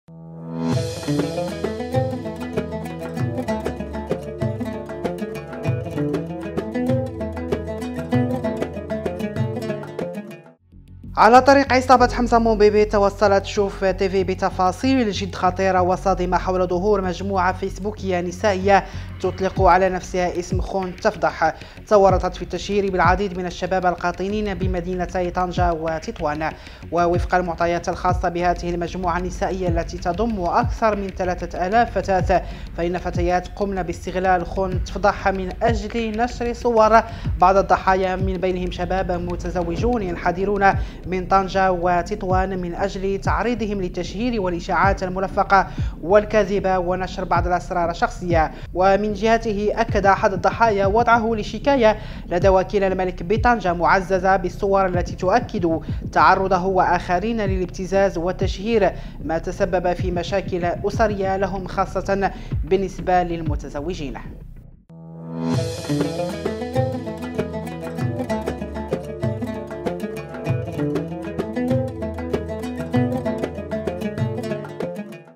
بعد "حمزة مون بيبي"..مجموعة نسائية تحت اسم “خون تفضح” تتورط في التشهير وابتزاز الشباب بتطوان وطنجة
على طريق عصابة "حمزة مون بيبي"، توصلت "شوف تيفي" بتفاصيل جد خطيرة وصادمة، حول ظهور مجموعة فيسبوكية نسائية تطلق على نفسها اسم "خون تفضح"، تورطت في التشهير بالعديد من الشباب القاطنين بمدينيتي طنجة وتطوان.